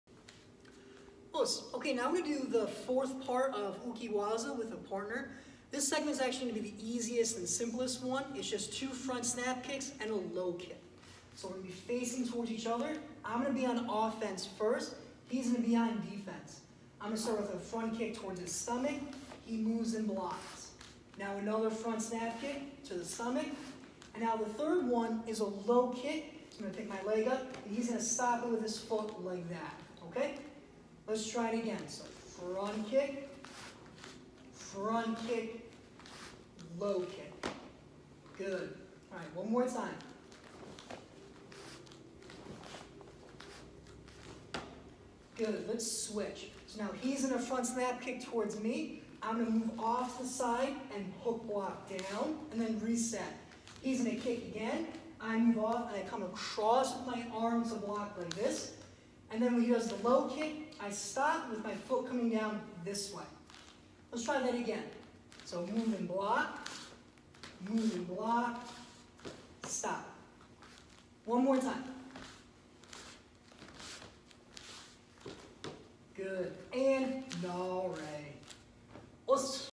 Uke Waza Part 4
Black belt partner drill part four with a partner